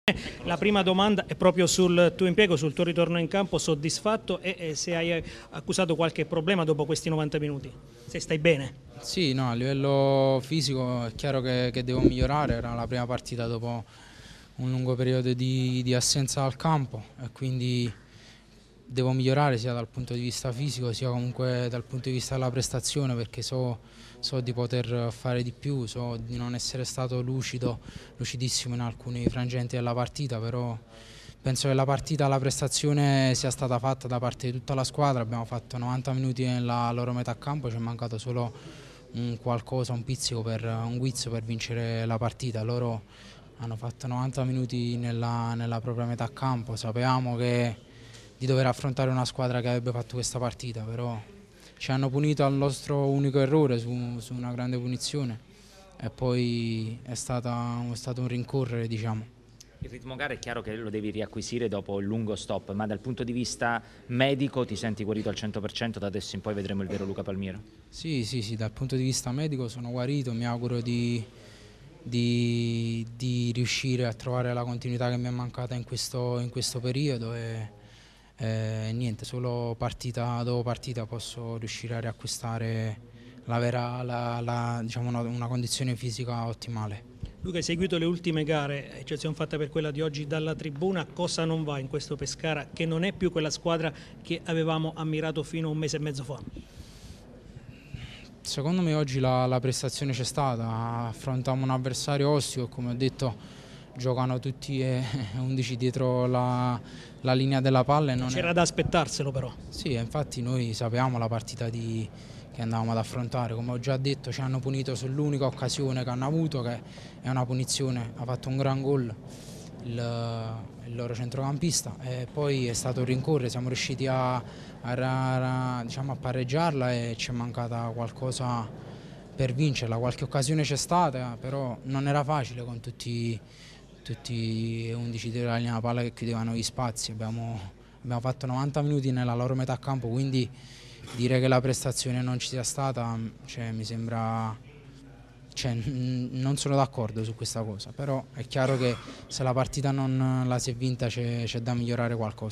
Pescara - Trapani 1-1: Luca Palmiero
Intervista posta gara al centrocampista del Pescara